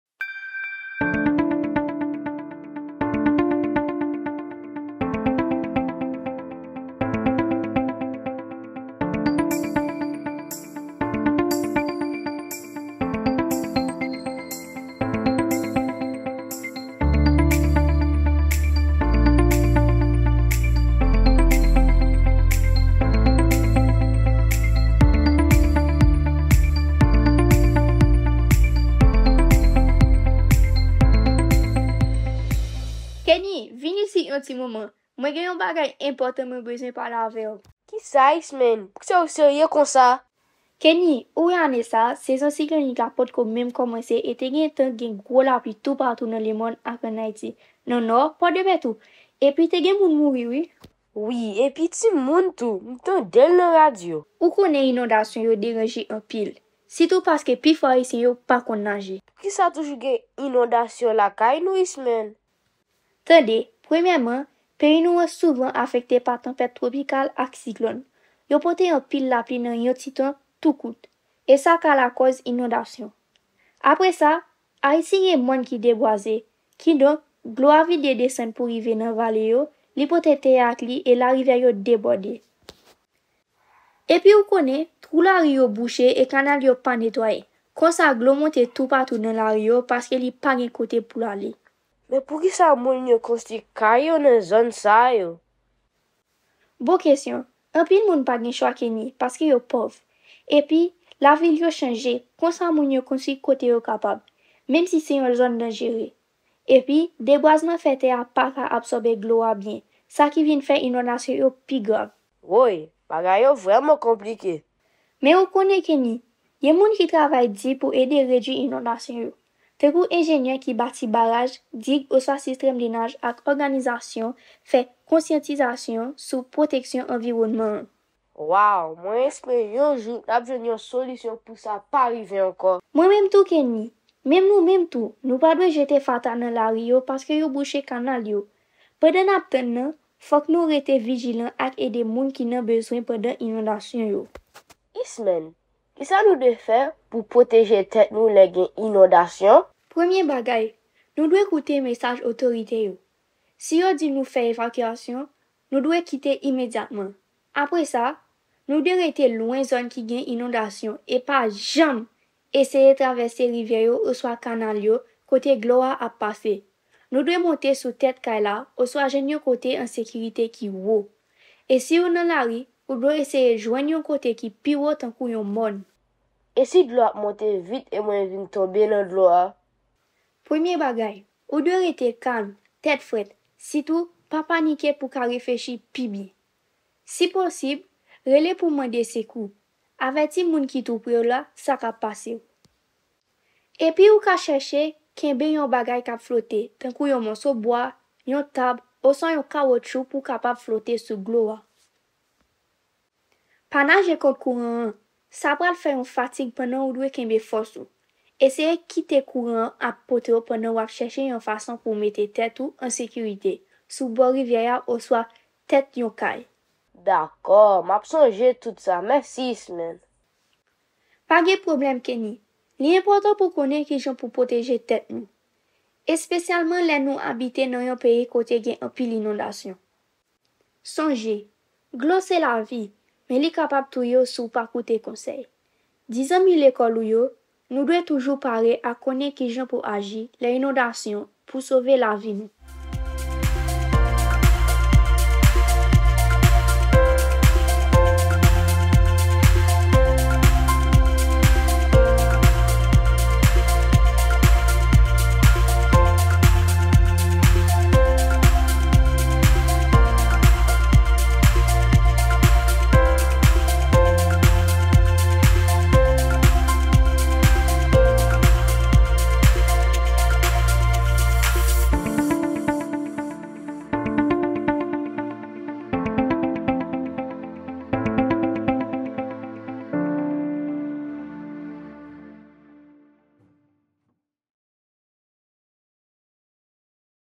Prevansyon GRD: Inondasyon
Kisa yon inondasyon ye? Inondasyon yo se youn nan katastwòf natirèl ki rive pi souvan. Chanjman klimatik ansanm ak aktivite imen ogmante risk monte dlo yo ak enpak yo. Videyo sa a ap aprann nou aksyon ki ka sove lavi lè gen inondasyon.

Incas Productions ap prezante nouvo videyo 2024 sou
pwogram edikasyon prevansyon ak konsyantizasyon sa a sou dezas natirel yo.